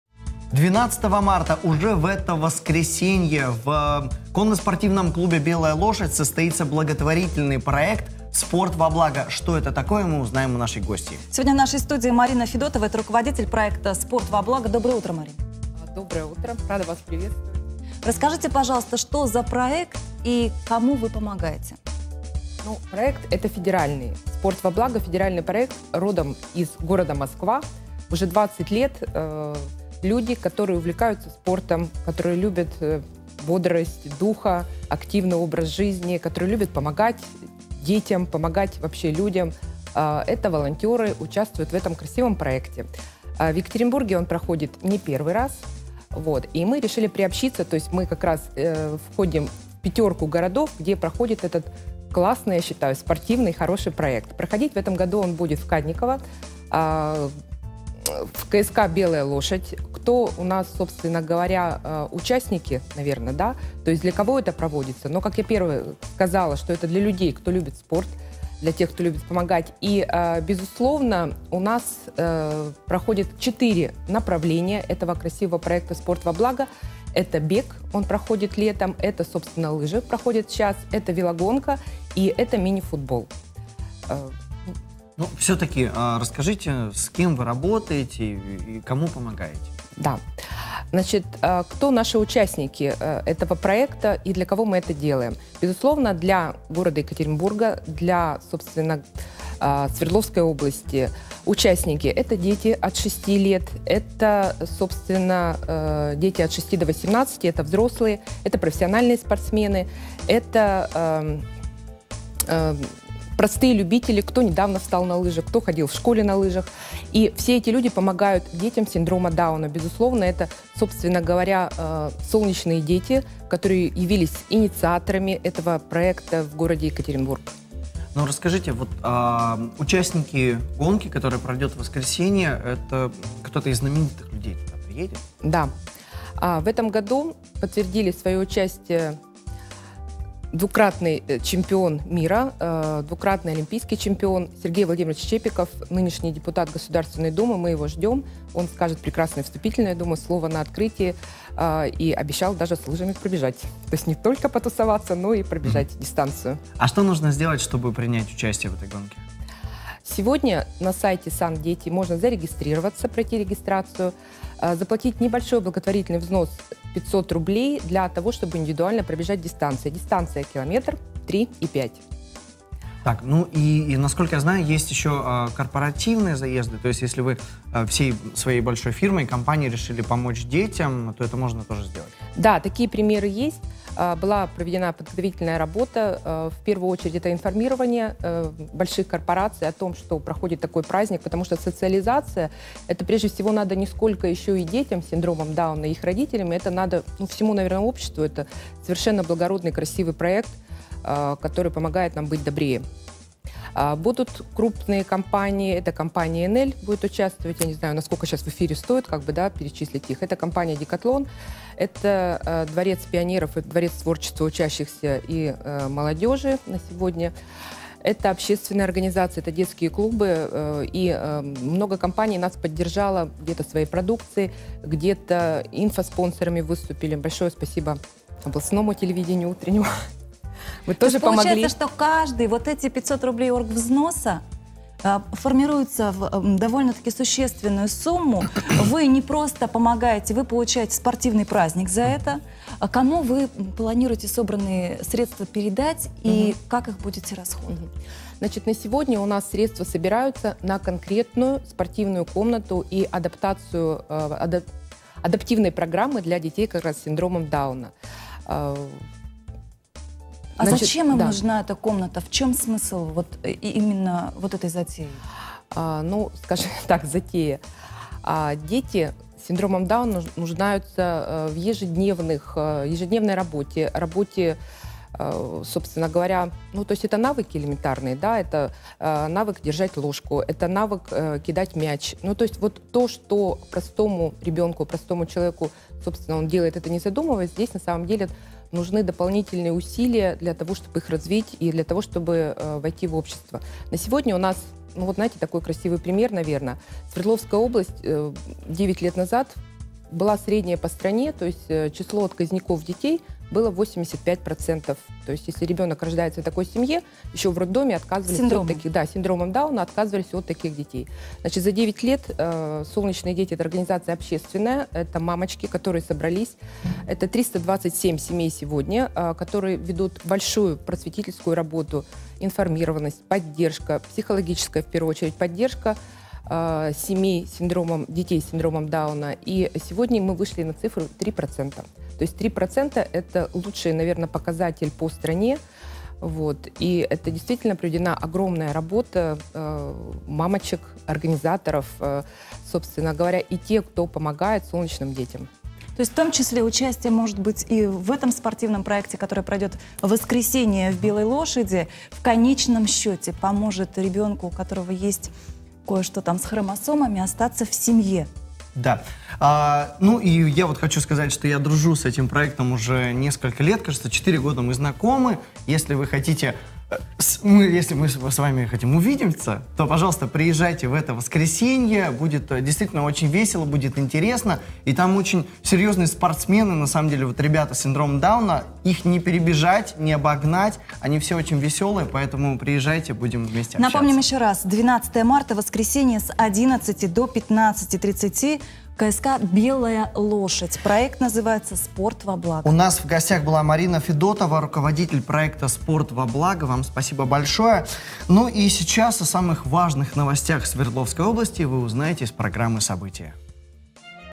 Спорт во благо
В это воскресенье в конноспортивном клубе "Белая лошадь" пройдёт лыжная гонка, которая поможет собрать средства в пользу детей с синдромом Дауна. Подробнее о мероприятии рассказывает наша гостья — руководитель проекта "Спорт во благо" Марина Федотова.